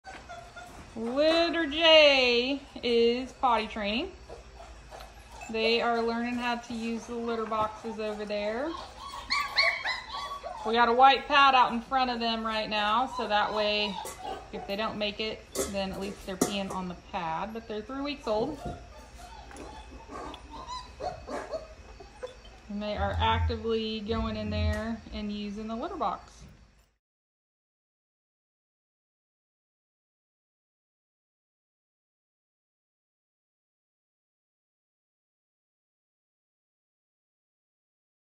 Puppy Culture Litter Box Training
AKC German Shepherd Puppies are learning how to use the litter box at 3 weeks old. At Salty Raccoon Ranch we teach our puppies early how to keep their own and bedding clean. This makes it easier to potty train your puppy.

Litter “J” was whelped on 7/15/22 and is 3 weeks old in this video.